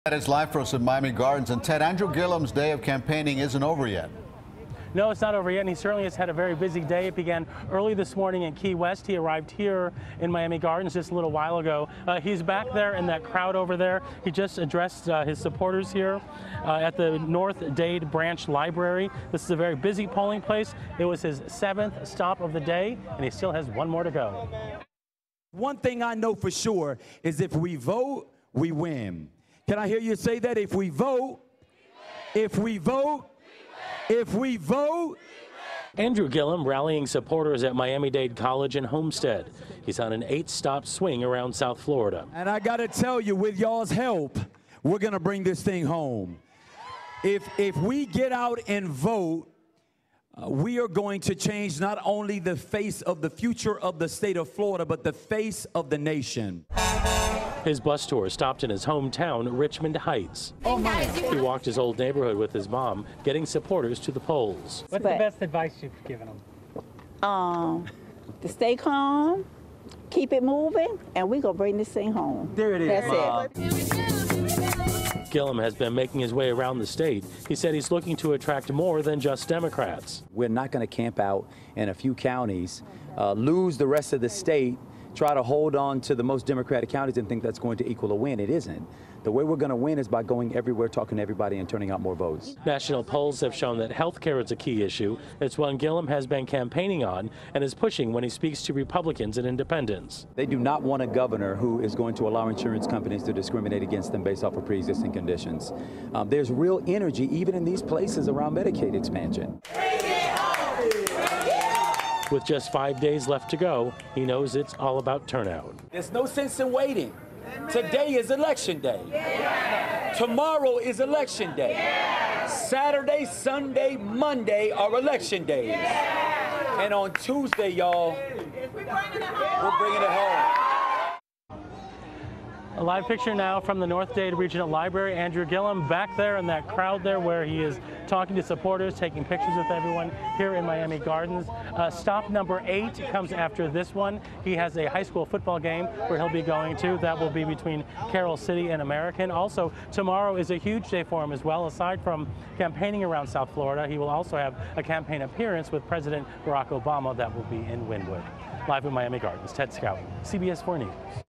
Andrew Gillum Campaigns In South Florida
Andrew Gillum, the Democratic candidate for governor, campaigned across South Florida on Thursday.